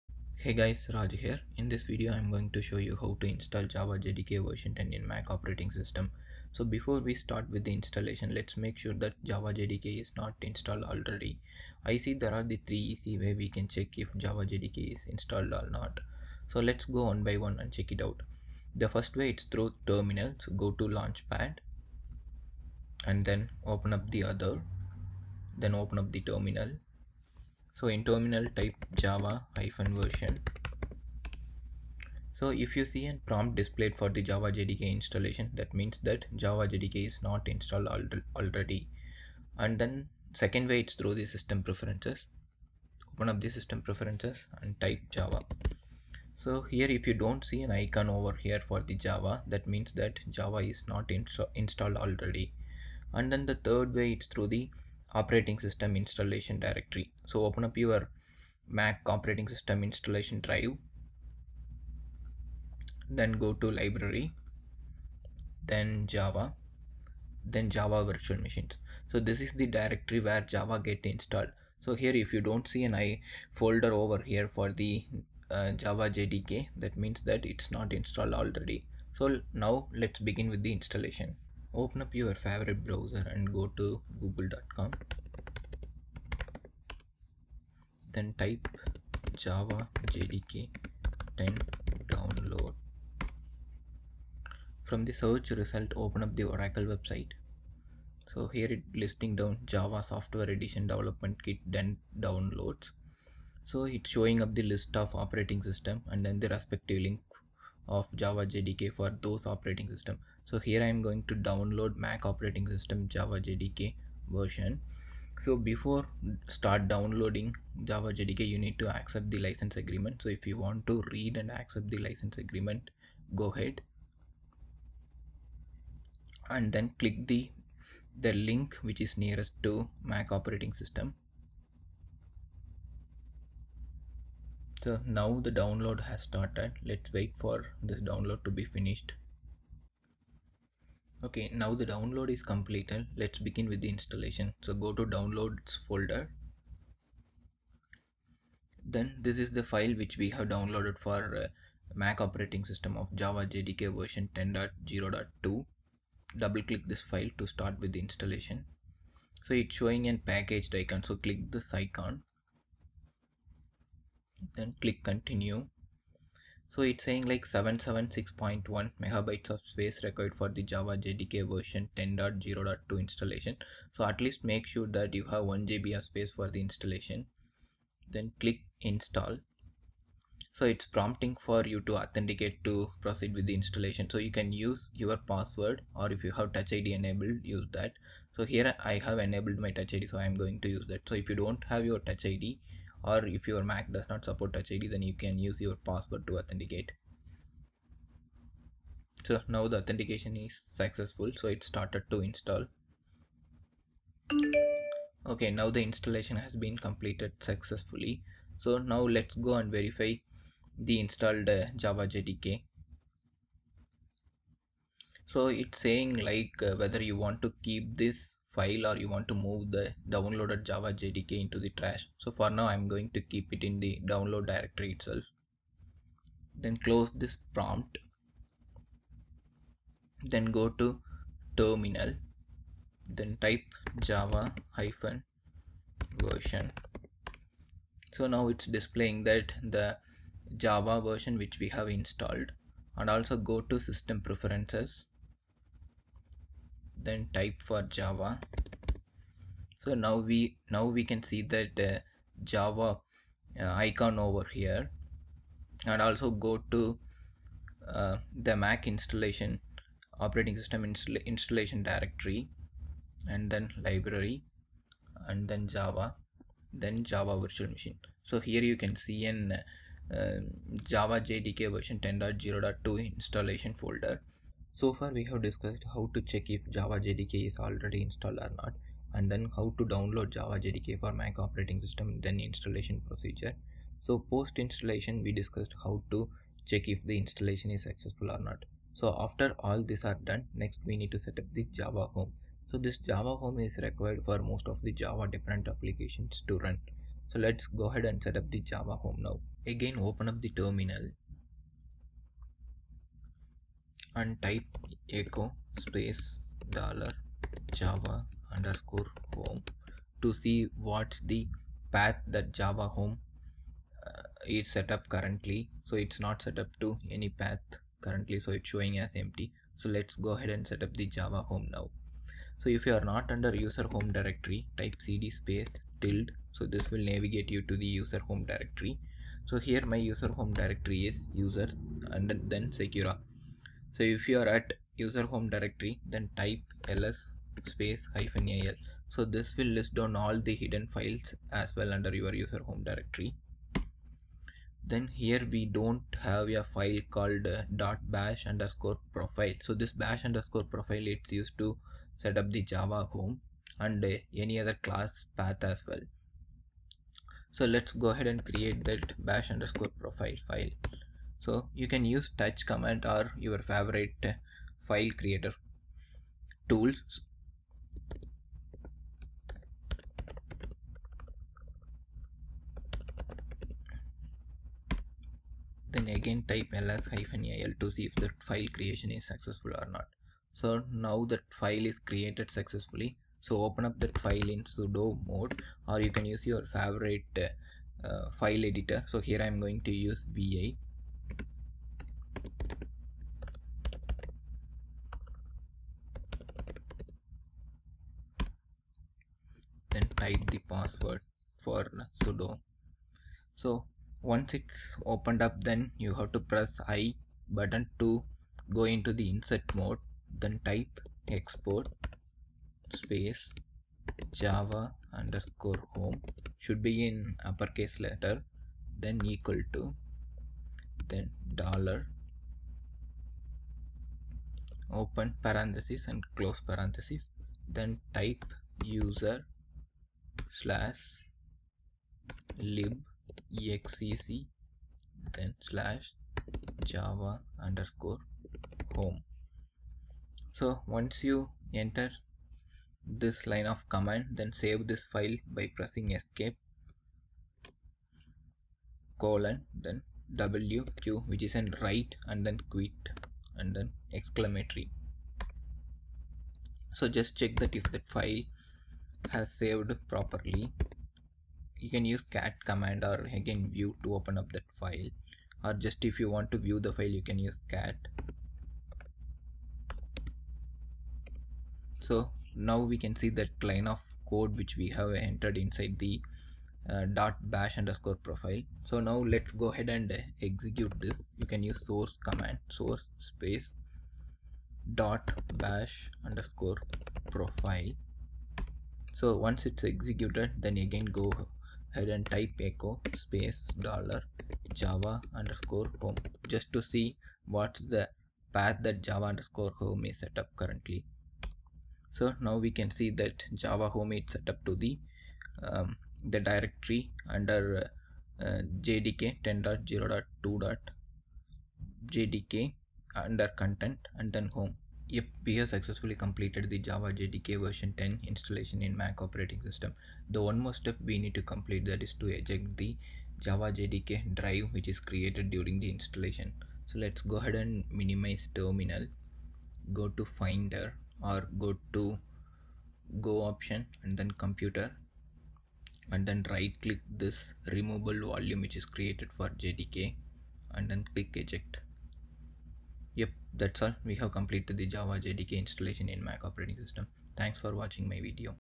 How to install Java JDK 10 with JAVA_HOME on Mac OS X?
This video will describe about How to install Java JDK(Java Development Kit) 10 with JAVA_HOME on Mac OS X.
Here are the commands used in Terminal.
1. Print Java version
 java -version
2. JAVA_HOME Setup
 a. Print JAVA_HOME Variable
  echo $JAVA_HOME
 b. Navigate to user home directory
  cd ~
 c. List all files/folders including hidden
  ls -al
 d. Create .bash_profile file
  touch .bash_profile
 e. Open .bash_profile file with vi in sudo mode
  sudo vi .bash_profile
 f. Insert mode in file to write
  Just press i button
 g. Set JAVA_HOME Path
  export JAVA_HOME=$(/usr/libexec/java_home)
 h. Save file
  Press Esc button and then :wq!
 i. Display .bash_profile file content
  cat .bash_profile
 j. Execute .bash_profile
  source .bash_profile